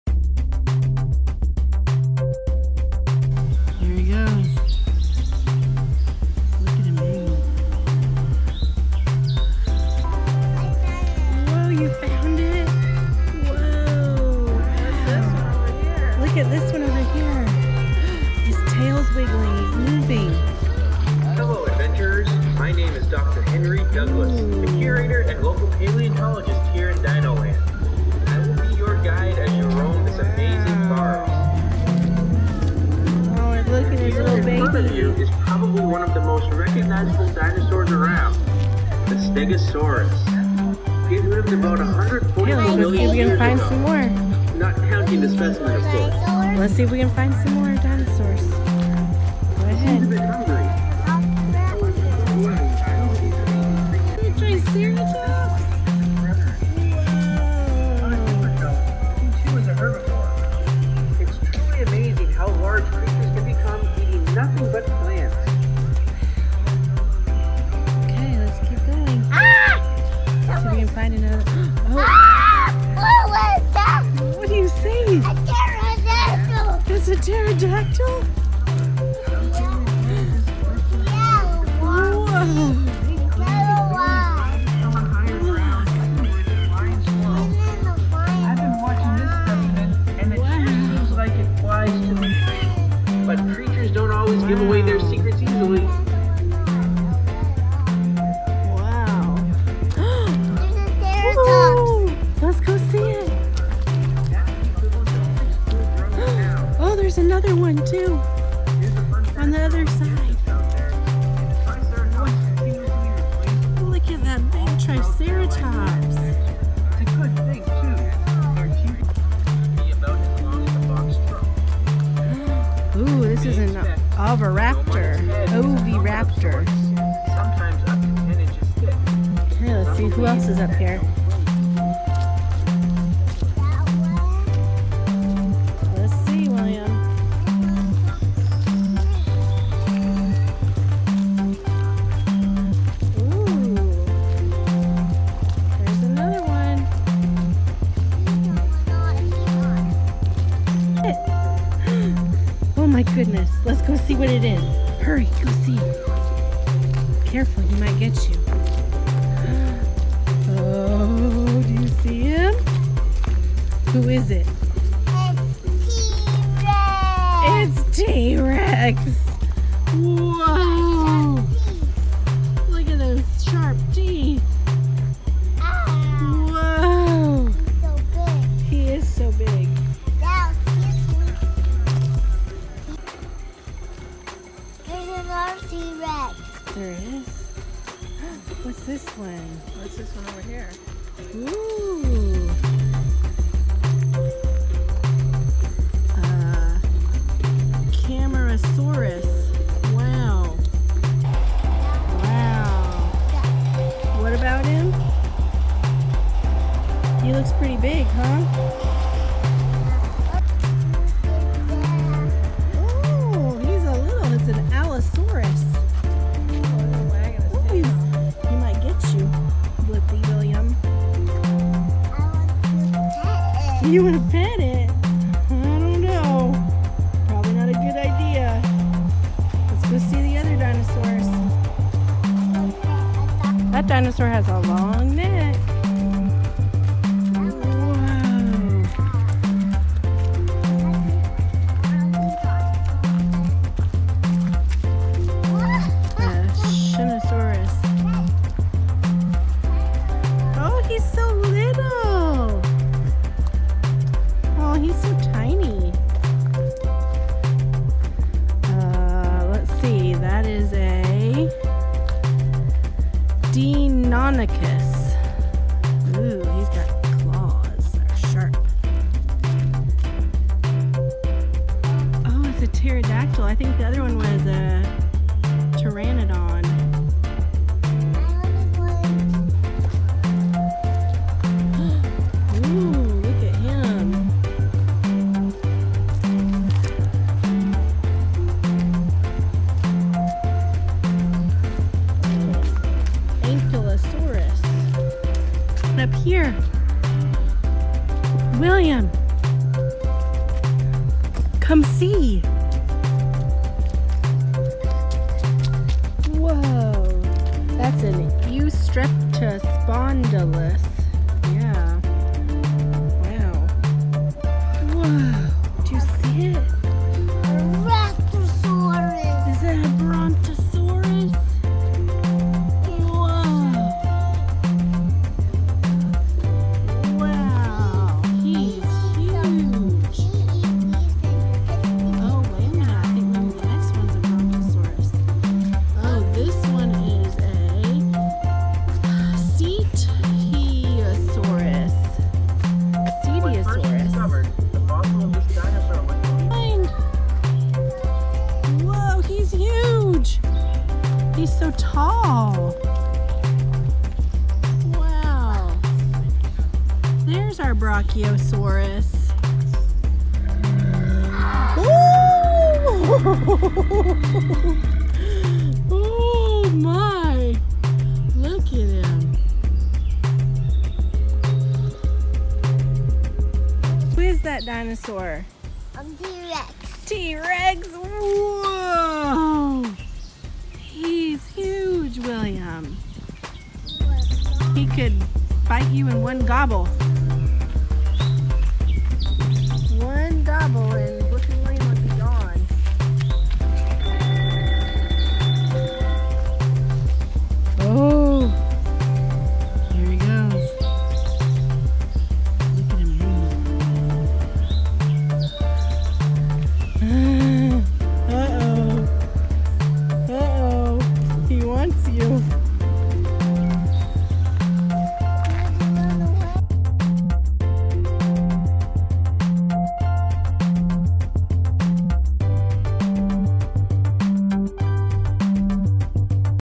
Dino land | Family Theme Park-rides & attractions
...Edaville Family Theme Park in Carver, Massachusetts. While it's not exclusively dinosaur-themed, it does have a section called Dino Land where kids can enjoy dinosaur-themed rides and attractions.
(AI generated description)